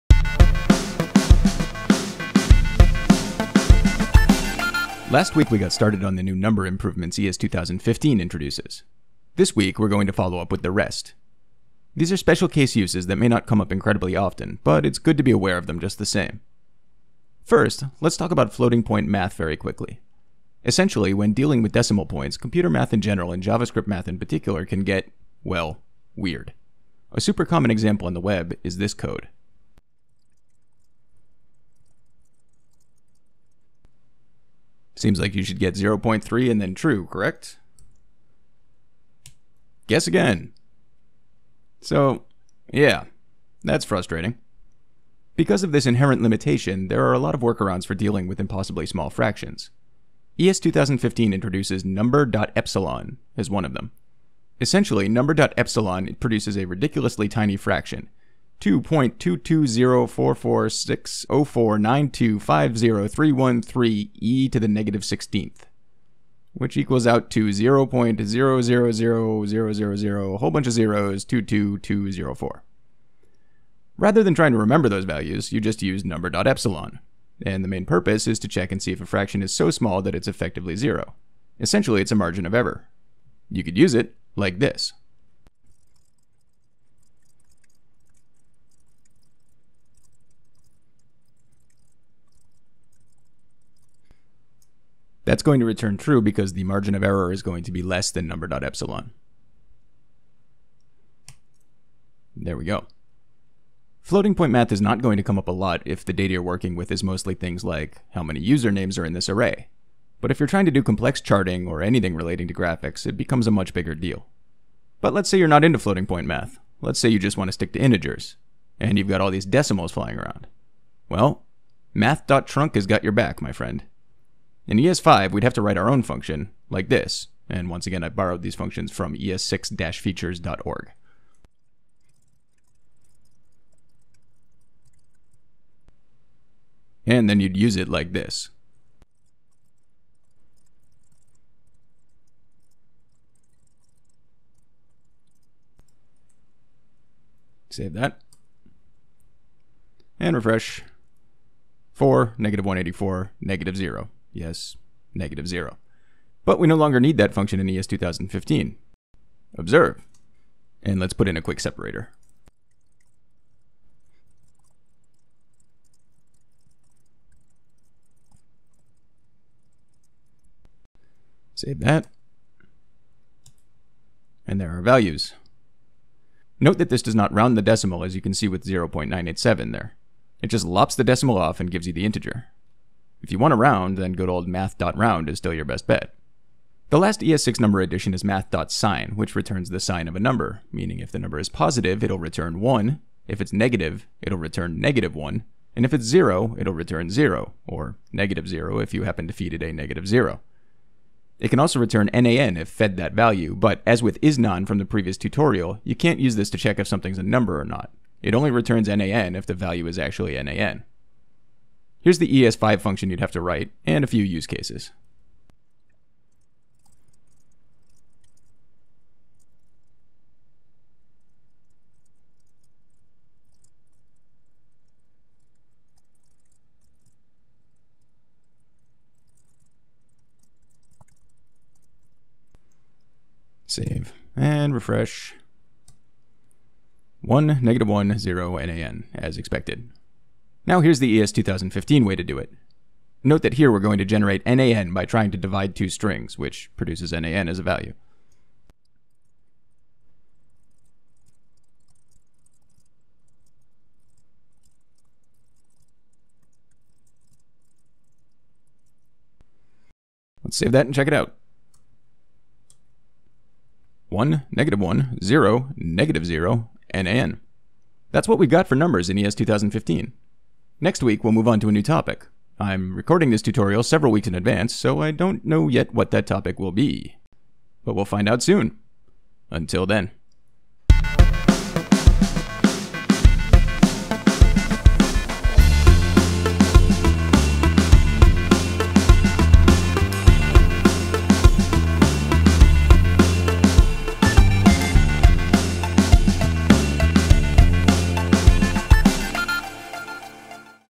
JS Quick Hits 28 - ES2015 Number Improvements Part 2
More numbers this week as we wrap up the quality of life improvements introduced in ES 2015.